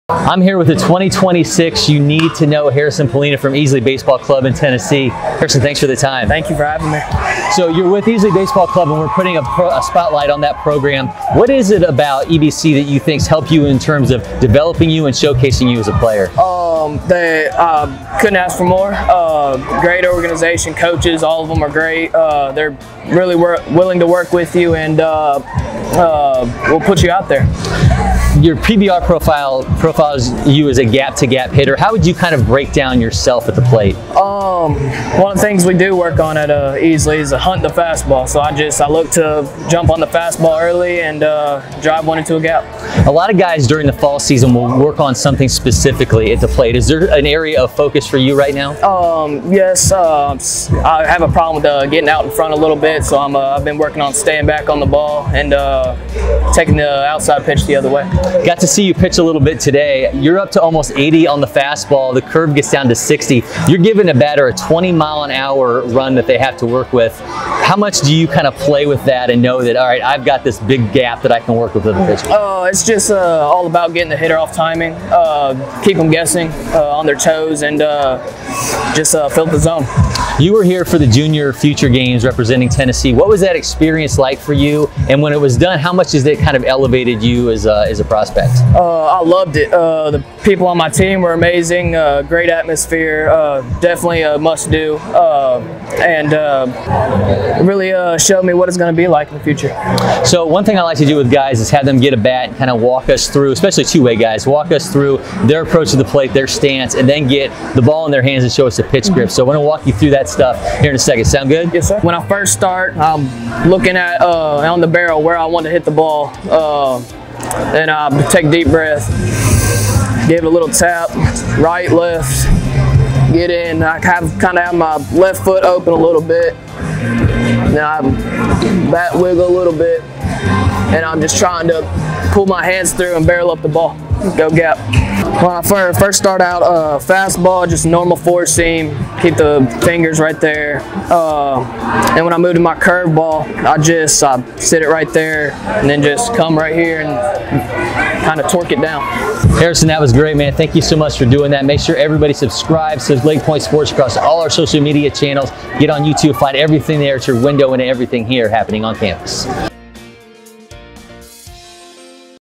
Get to know Easley Baseball Club 2026 outfielder/pitcher Harrison Pollina
A Prep Baseball Report Junior Future Games participant who can do damage at the plate and on the mound, Easley Baseball's Harrison Pollina (Munford Middle School) is a 2026 you need to know.

He catches up with Cory McCartney.